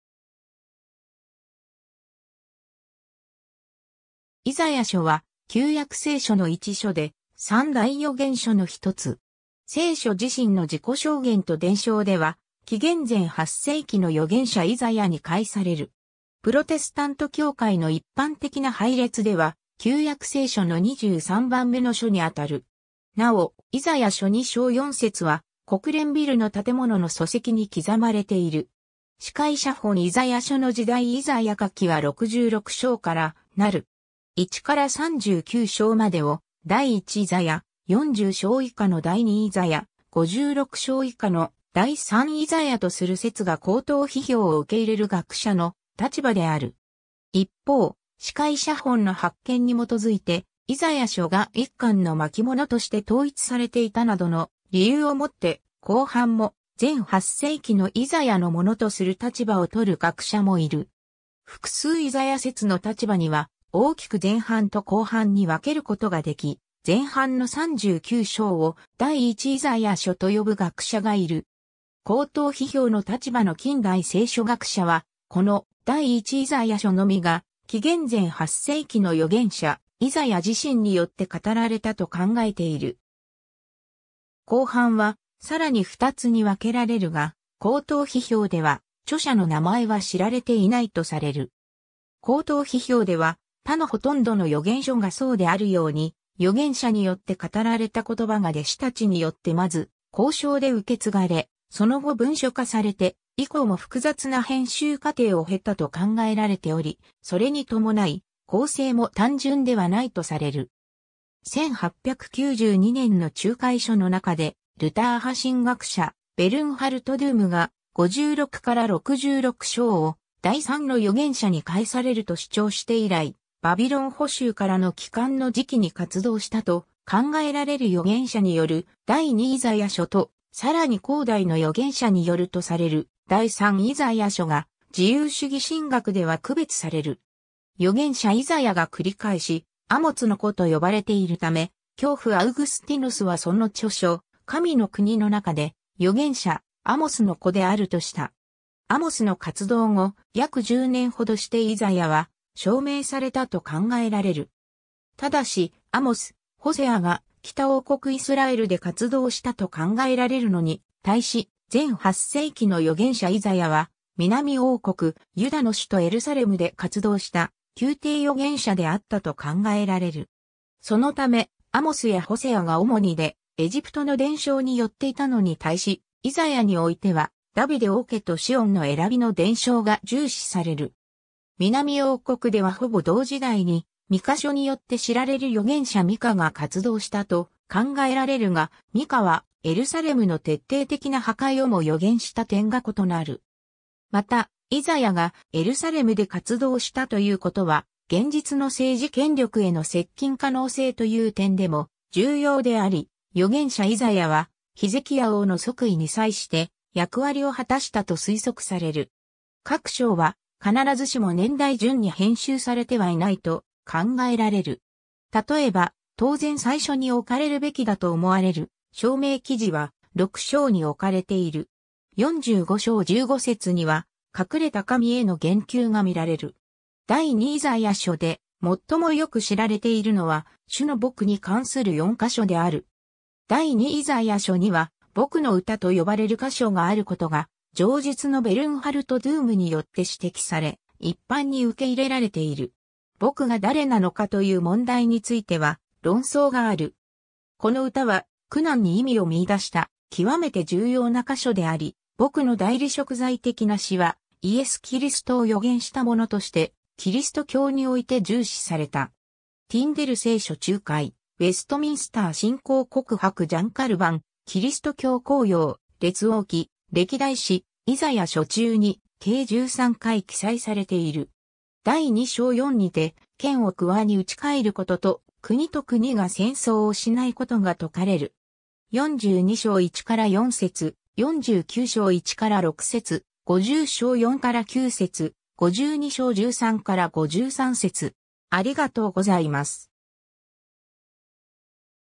イザヤ書
『イザヤ書』（イザヤしょ）は、旧約聖書の一書で、三大預言書（『イザヤ書』、『エレミヤ書』、『エゼキエル書』）の一つ。
聖書自身の自己証言と伝承では紀元前8世紀の預言者イザヤに帰される。
プロテスタント教会の一般的な配列では旧約聖書の23番目の書にあたる。
なお、『イザヤ書』2章4節は、国連ビルの建物の礎石に刻まれている。
 死海写本 イザヤ書の時代 イザヤ書は66章からなる。
1-39章までを第一イザヤ、40章以下の第二イザヤ、56章以下の第三イザヤとする説が高等批評を受け入れる学者の立場である。
一方、死海写本の発見に基づいて、『イザヤ書』が一巻の巻物として統一されていたなどの理由をもって、後半も前8世紀のイザヤのものとする立場をとる学者もいる。
 複数イザヤ説の立場には、大きく前半と後半に分けることができ、前半の39章を『第一イザヤ書』と呼ぶ学者がいる。
高等批評の立場の近代聖書学者は、この『第一イザヤ書』のみが紀元前8世紀の預言者イザヤ自身によって語られたと考えている（ただし、高等批評では1-39章においても多くの箇所が前8世紀のイザヤ自身によるものではないと考えられる）。
 後半はさらに2つに分けられるが、高等批評では著者の名前は知られていないとされる。
 高等批評では、他のほとんどの預言書がそうであるように、預言者によって語られた言葉が弟子たちによってまず口承で受け継がれ、その後文書化されて以降も複雑な編集過程を経たと考えられており、それに伴い、構成も単純ではないとされる。
 1892年の注解書の中でルター派神学者ベルンハルト・ドゥーム（Bernhard Duhm）が、56-66章を第三の預言者に帰されると主張して以来、バビロン捕囚からの帰還の時期に活動したと考えられる預言者による『第二イザヤ書』（40～55章）とさらに後代の預言者によるとされる『第三イザヤ書』（56～66章）が自由主義神学では区別される。
 預言者イザヤが繰り返し「アモツの子」と呼ばれているため、教父アウグスティヌスはその著書『神の国』の中で、預言者アモスの子であるとした。
アモスの活動後約10年ほどしてイザヤは召命されたと考えられる。
ただし、アモス、ホセアが北王国イスラエルで活動したと考えられるのに対し、前8世紀の預言者イザヤは、南王国ユダの首都エルサレムで活動した宮廷預言者であったと考えられる。
そのため、アモスやホセアが主に出エジプトの伝承に拠っていたのに対し、イザヤにおいては、ダヴィデ王家とシオン（エルサレム）の選びの伝承が重視される。
南王国ではほぼ同時代にミカ書によって知られる預言者ミカが活動したと考えられるが、ミカはエルサレムの徹底的な破壊をも預言した点が異なる。
 また、イザヤがエルサレムで活動したということは、現実の政治権力への接近可能性という点でも重要であり、預言者イザヤは、ヒゼキヤ王の即位に際して役割を果たしたと推測される。
 各章は、必ずしも年代順に編集されてはいないと考えられる。
例えば、当然最初に置かれるべきだと思われる召命記事は、6章に置かれている。
 45章15節には、「隠れた神」（ラテン語訳では Deus absconditus）への言及が見られる。
 『第二イザヤ書』で最も良く知られているのは、「主の僕（しもべ）」（42:1～4、 49:1～6、 50:4～9、52:13～53:12）に関する4箇所である。
『第二イザヤ書』には「僕（しもべ）の歌」と呼ばれる箇所があることが、上述のベルンハルト・ドゥームによって指摘され、一般に受け入れられている。
「僕（しもべ）」が誰なのかという問題については論争がある。
この歌は、苦難に意味を見出した極めて重要な箇所であり、「僕（しもべ）」の代理贖罪的な死は、イエス・キリストを預言したものとしてキリスト教において重視された。
 ^ 『ティンデル聖書注解』 ^ 『ウェストミンスター信仰告白』 ^ ジャン・カルヴァン『キリスト教綱要』 ^ 列王記、歴代誌、イザヤ書中に計13回記載されている。
 ^ 第2章4にて「剣を鍬に打ちかえること」と国と国が戦争をしないことが説かれる。
 ^ 42章1-4節、49章1-6節、50章4-9節、52章13-53節。